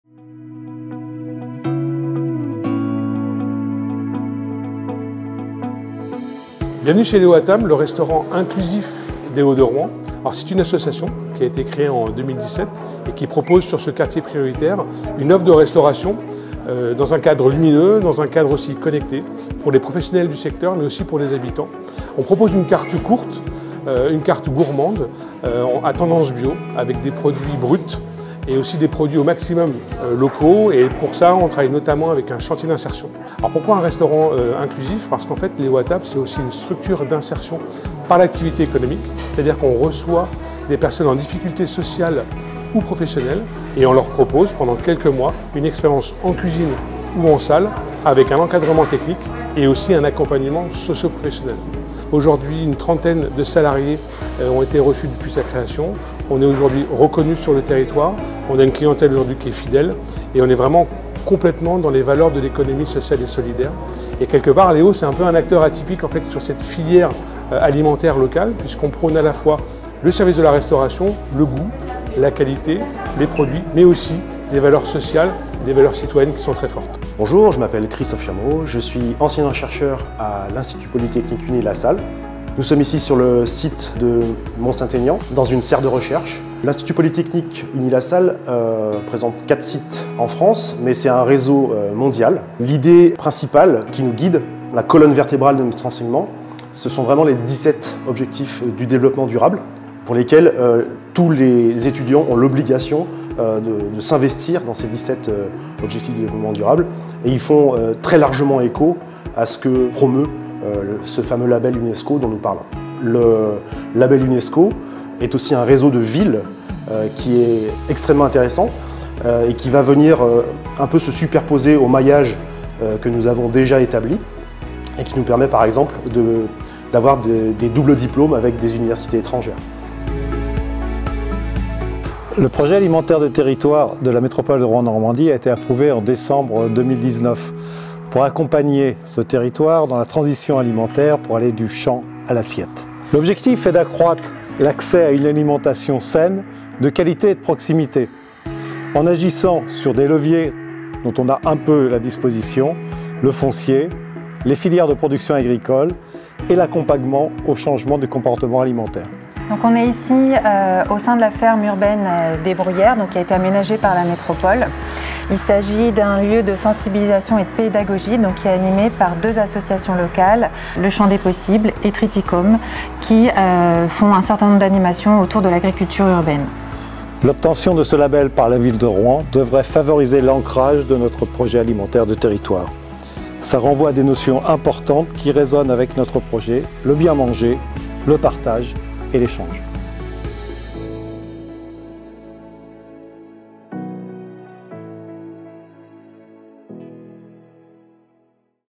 Rouen, ville créative Unesco catégorie Gastronomie - "Transition Écologique et Économie Solidaire"
Rouen est la première et seule ville française, pour l'heure, à avoir intégré le réseau Unesco "Ville créative" dans le domaine de la gastronomie en novembre 2021.
Ce Réseau international vise à renforcer la coopération entre les villes ayant identifié la créativité comme un facteur stratégique de développement durable aux niveaux économique, social, culturel et environnemental.
Afin d’atteindre les objectifs spécifiés dans l’énoncé de mission du Réseau des Villes créatives, et ainsi faire de la créativité un levier de développement urbain durable, Rouen possède un plan d'action en 6 volets.
Voici une vidéo de présentation du volet "Transition Écologique et Économie Solidaire".